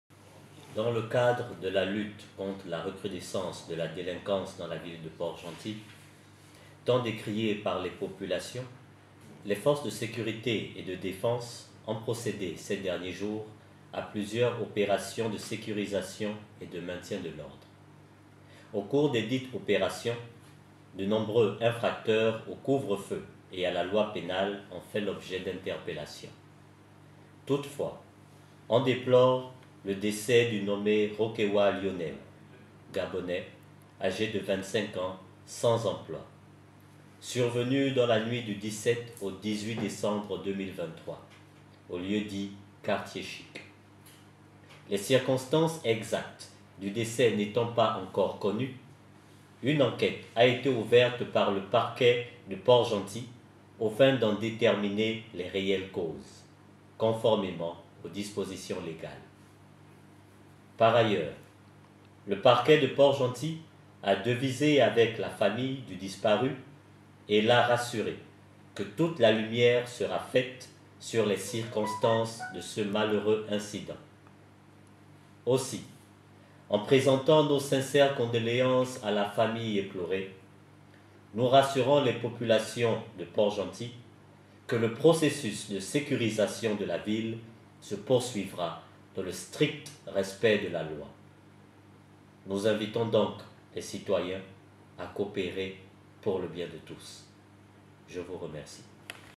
à Port-Gentil communiqué du procureur de la République
Bonjour et bienvenue sur ma chaîne dans cet extrait, le communiqué du procureur de la République Pierre J. Aperano Essongue portant sur le décès d'un compatriote survenu dans la commune de Port-Gentil.

Le procureur de la République ouvre une enquête sue les circonstances qui sont à l'origine du décès de ce jeune compatriote.

Puisse son âme reposer en paix 🙏🏼🙏🏼